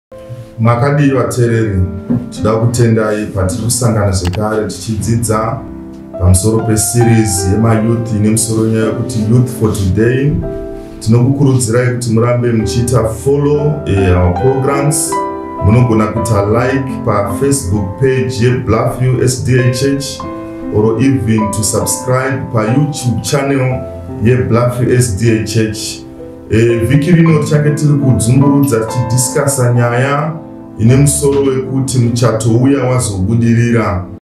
Youth For Today || Muchato Uya Wazobudirira Pt 1 || 19 December 2020 || Promo
Join us for Youth For Today.
With Pastor Mlambo and Team on "Muchato Uya Wazobudirira Pt 1".
at 1515.